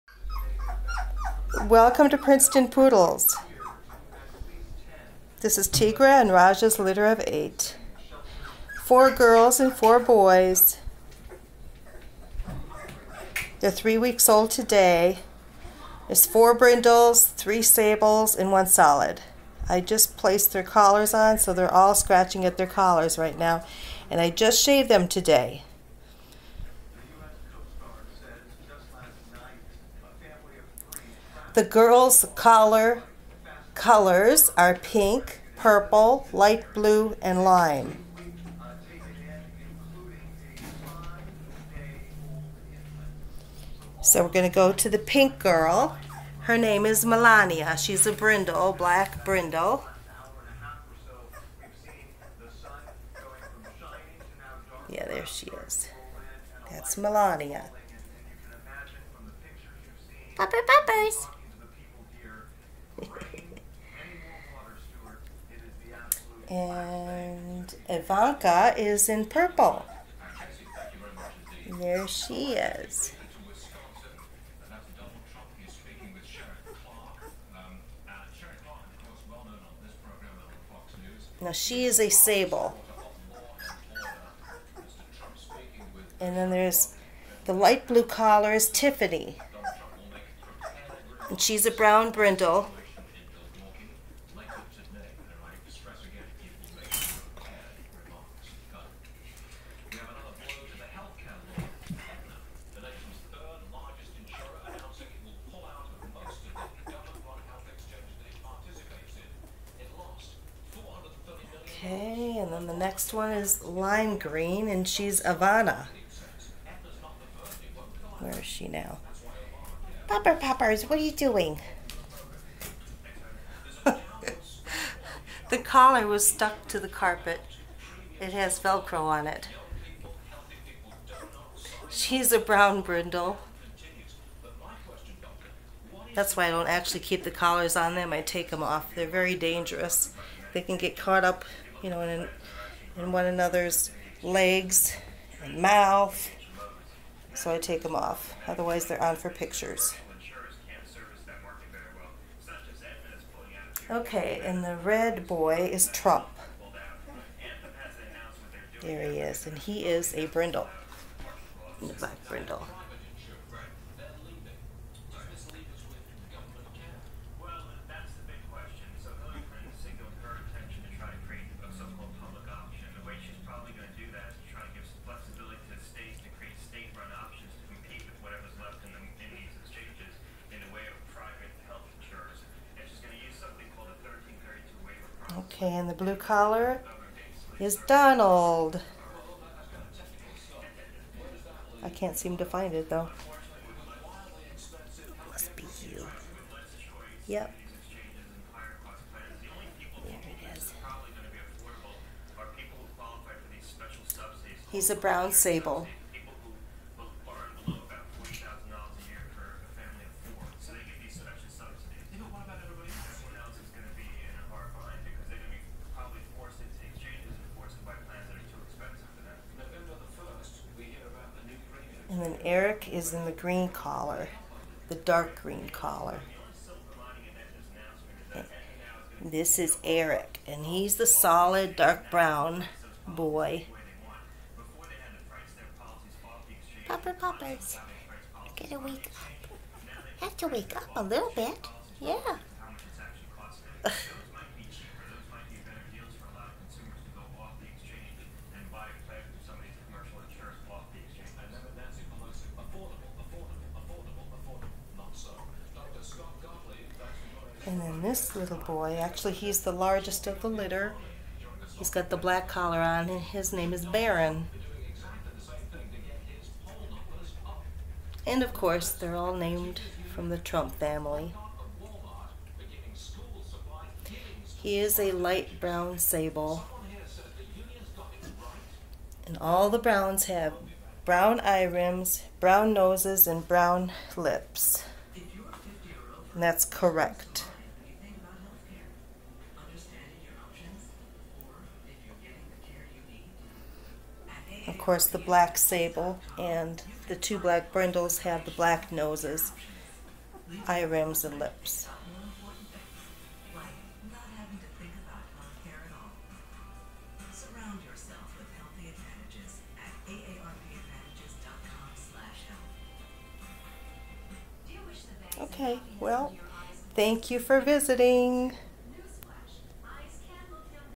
Tigre' x Raja Std. Poodle pups born 08/26/16
AKC Standard poodle puppies at 3 weeks old.  4 girls and 4 boys.  2 black brindles, 2 brown brindles, 1 black sable, 2 brown sables and 1 dark brown solid color.  Both parents are AKC Registered.  They are full body brindle UKC Champions and fully genetically health tested.